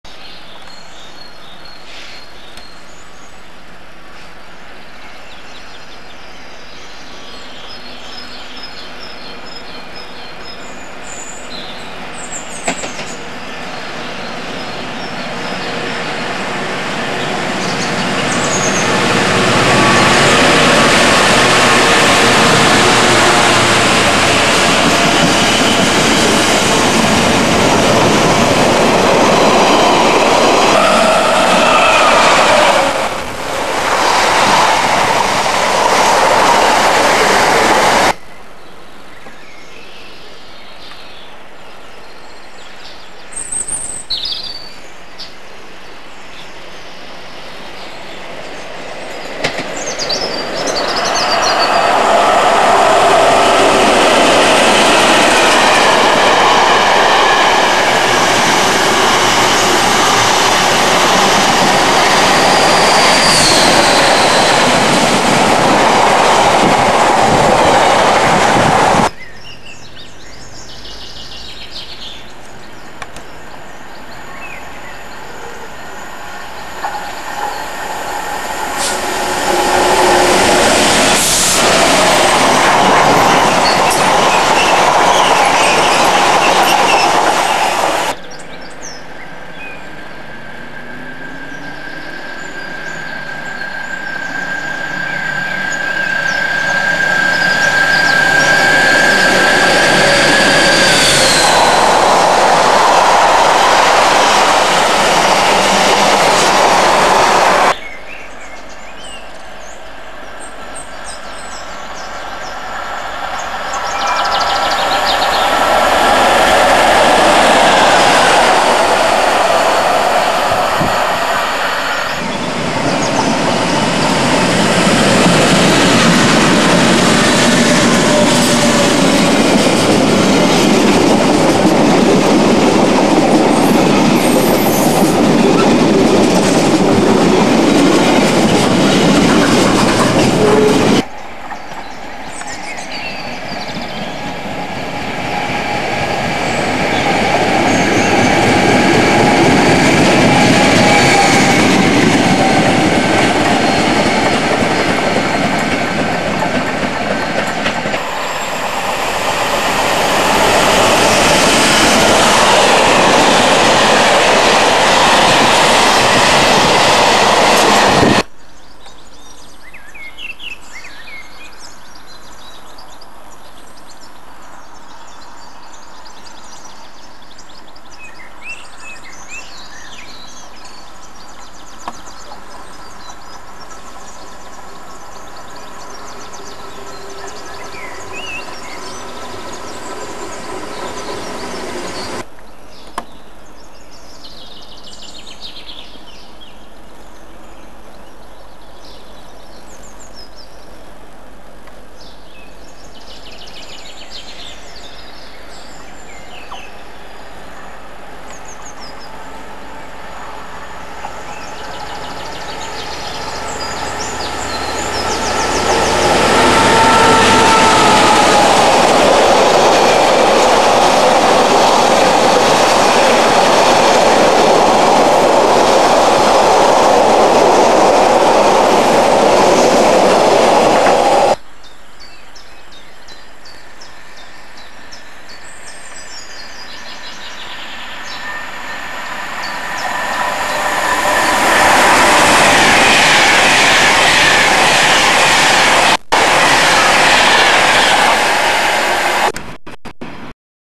ACTON BRIDGE JUNE 2005
AN EARLY MORNING VISIT TO THE STATION,CLASS 66S,57S,86S,90